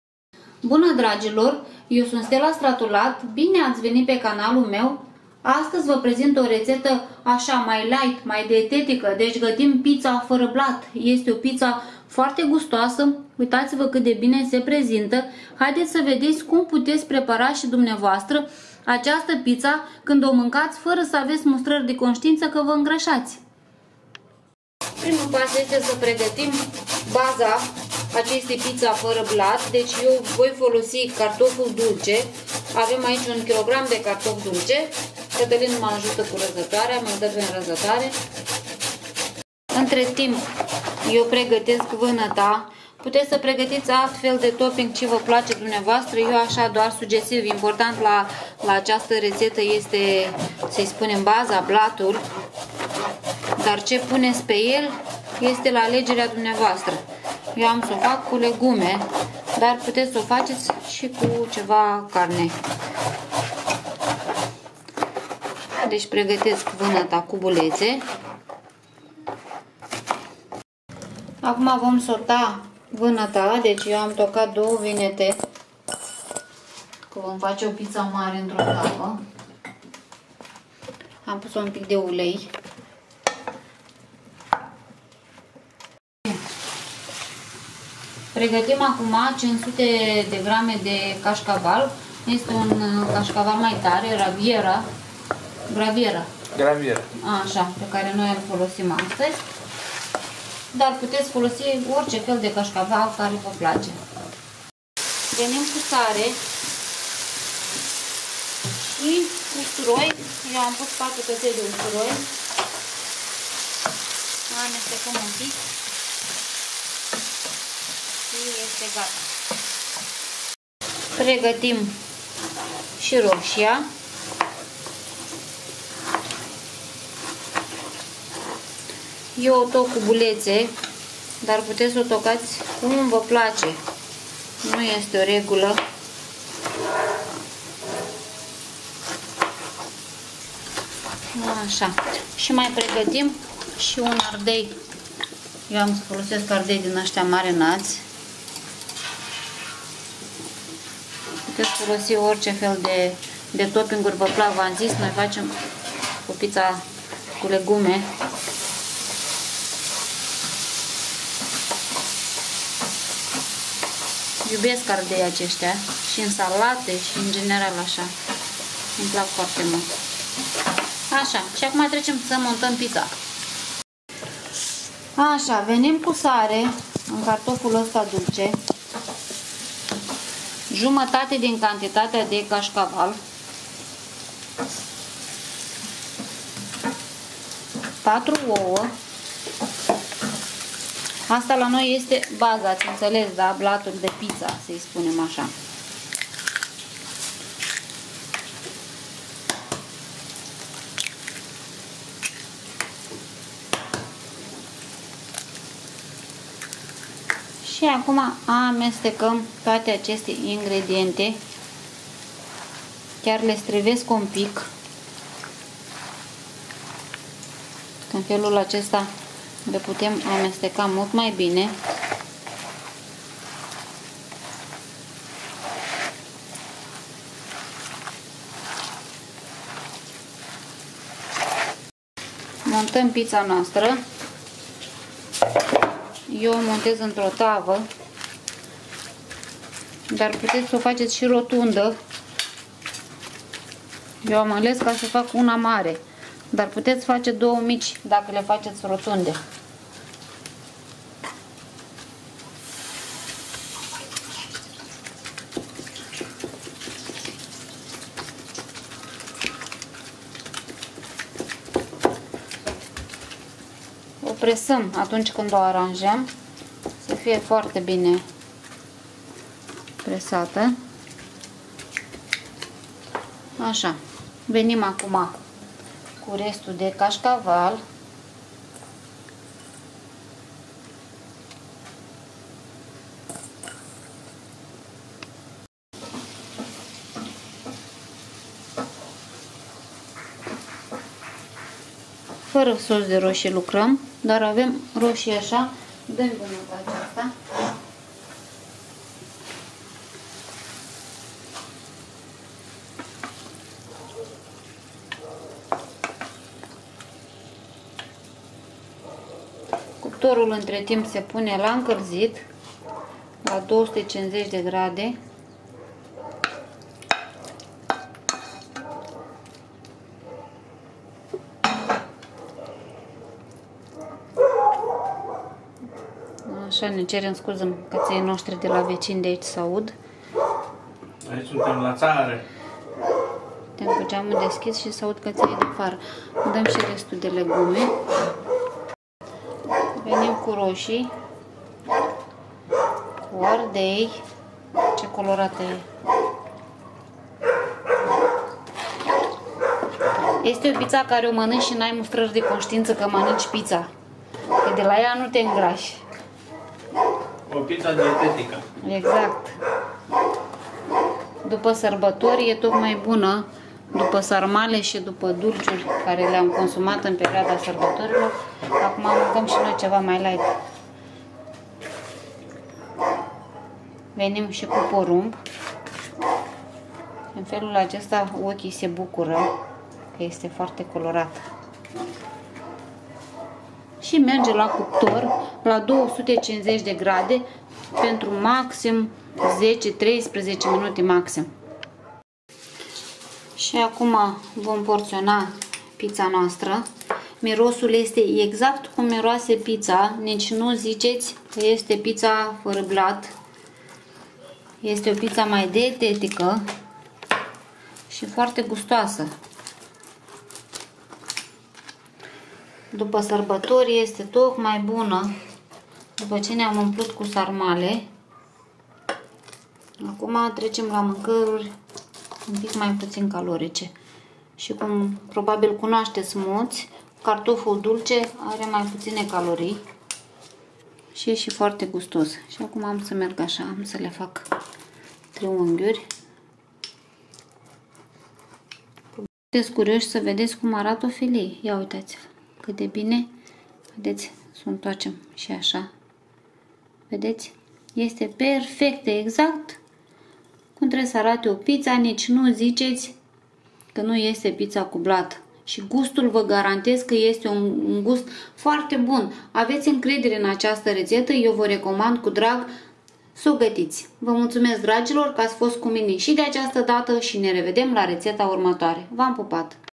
Cea mai buna pizza fara blat.
cartofi dulci 1kg
cascaval 500g
4 oua
1 rosie
1 ardei
2 vinete
Pizza 100g are 117kcal